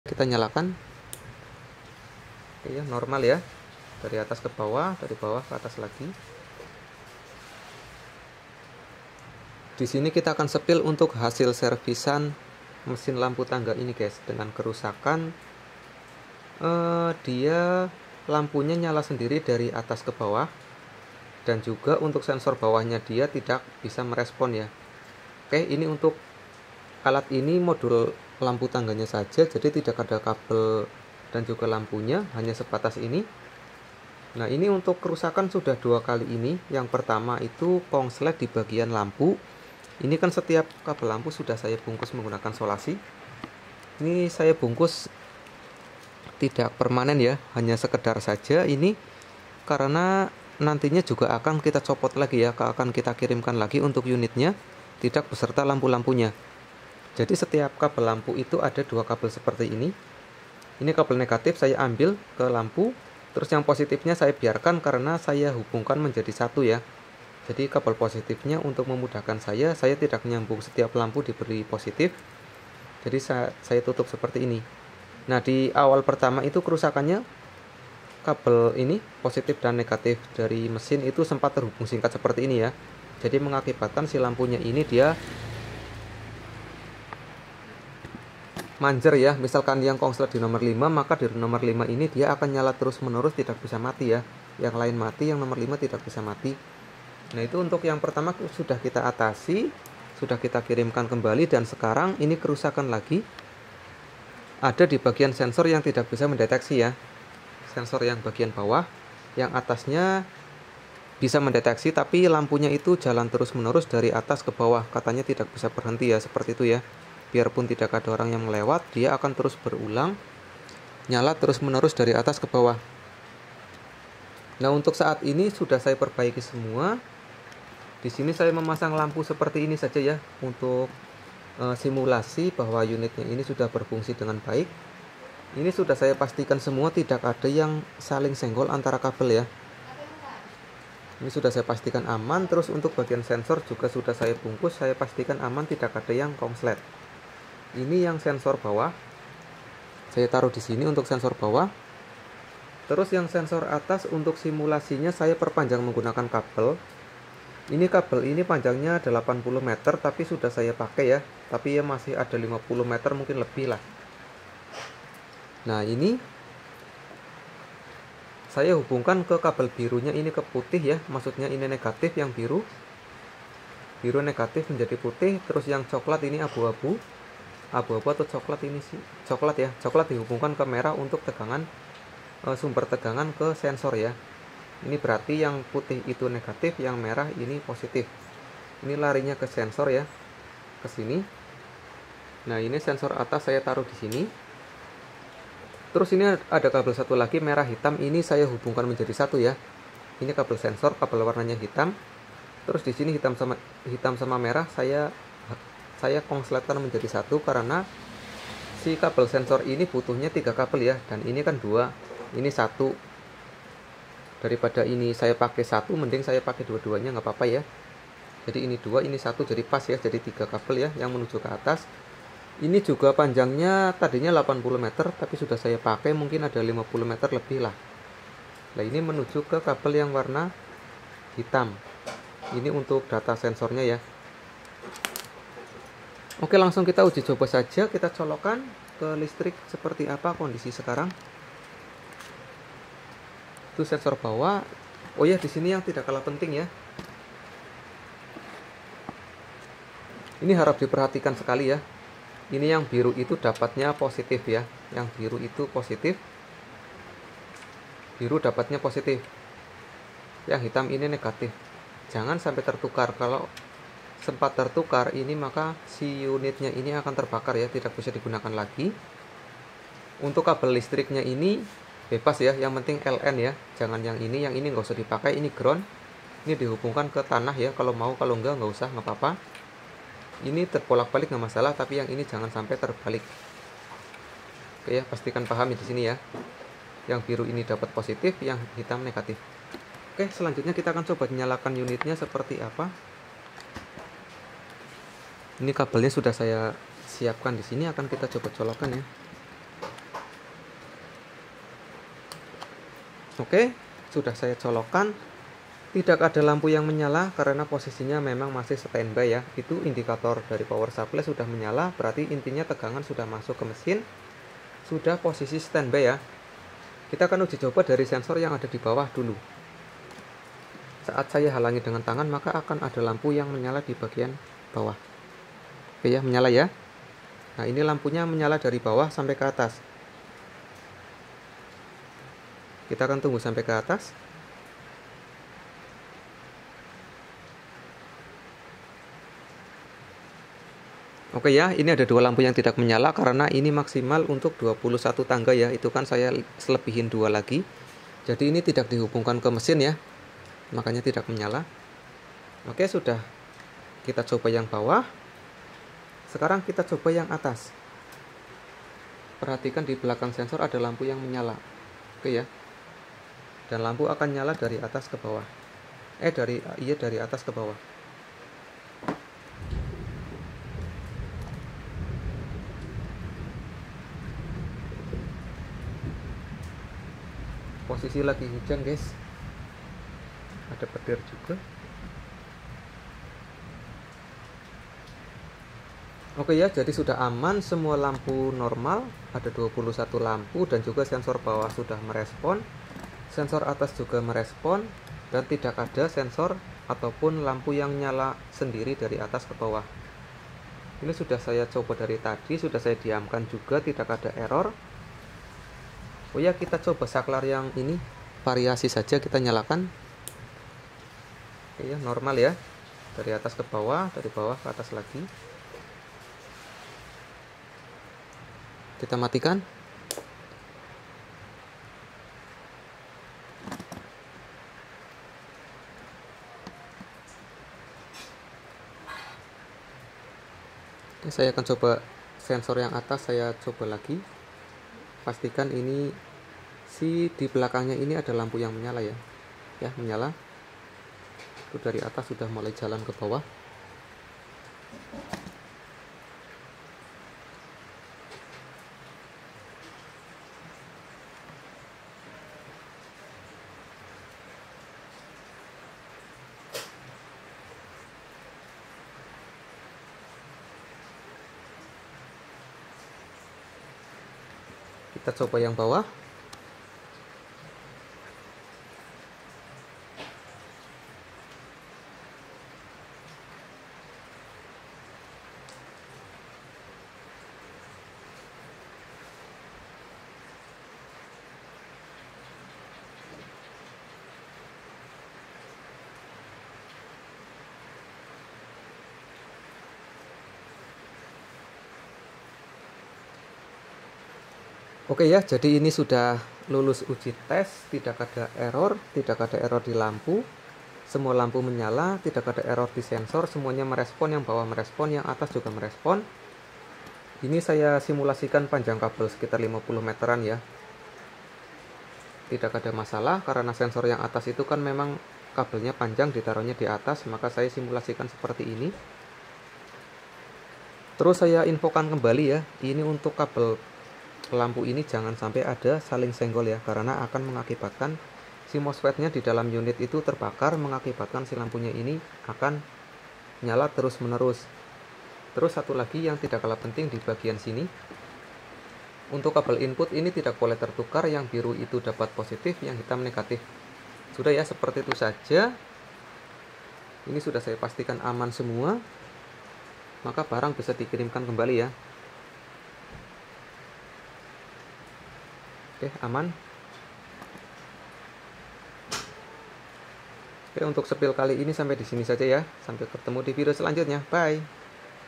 review servis mesin lampu led tangga sensor gerak
review servis mesin lampu tangga sensor dengan kerusakan nyala terus menerus dari atas kebawah dan sensor bawah tidak merespon, sudah diatasi semua

terimakasih